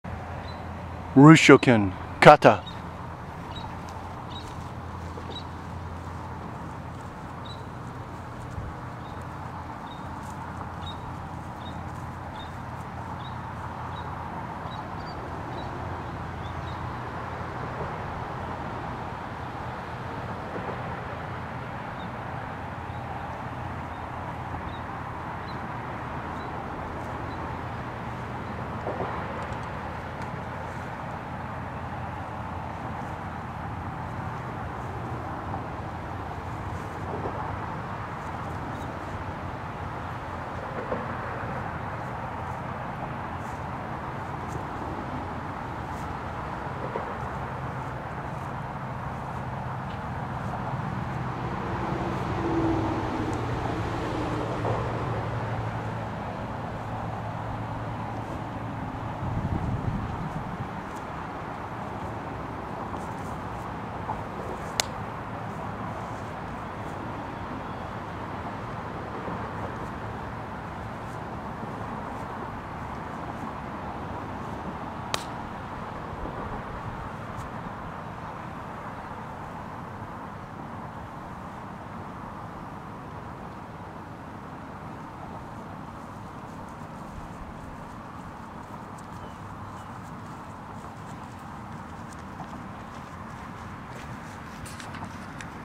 Ryushoken Kata
Translated in several ways by many people.  I was first taught it as "First Dragon Fist" when I learned it in the late 70s and early 80s while taking Shorin-Ryu and Twin Dragons Kung Fu